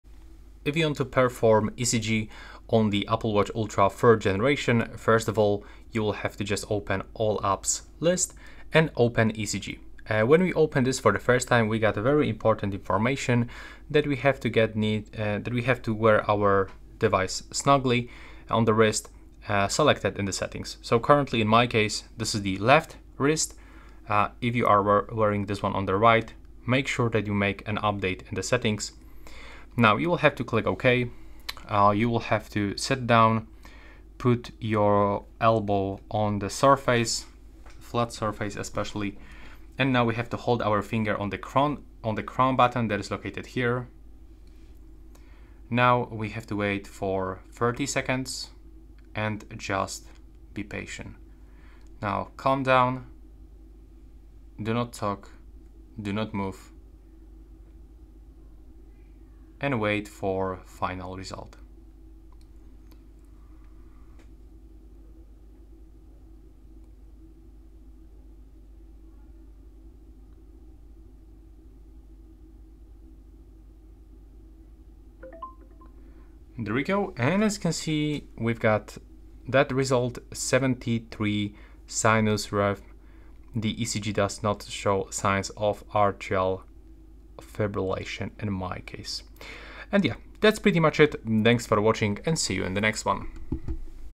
Apple Watch Ultra 3: How to do ECG
This video provides a quick guide on how to do an ECG with the Apple Watch Ultra 3. It covers finding and opening the ECG app, holding your finger on the Digital Crown without pressing it, and keeping your wrist steady during the 30-second recording. Learn how to view, save, and share your ECG results using the Apple Health app for heart health monitoring with the Ultra 3.

Greetings,
Dave from HowToDevices :)